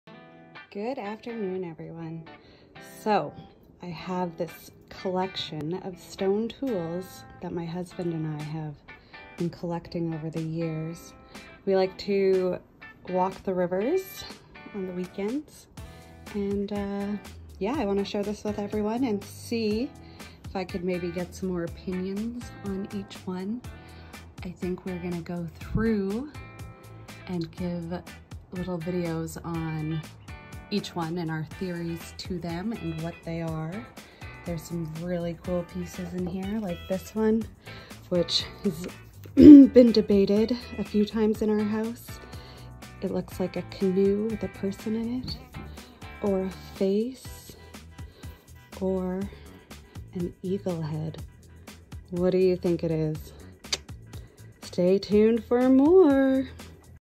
Indigenous Stone Tools found in Alberta, Canada Rivers.
A collections of Indigenous Stone tools found throughout Alberta. We have stone hammers, an effigy, stone carving tools, nutting stones and grinding tools. We are curious to find more information on these artifacts and would love to get input from others with knowledge on Native American stone tools specifically found in the area. Some of them we know are tools but not exactly sure what kind. Some of them are dual purpose and one has drawings burnt on it in resin like cave drawings.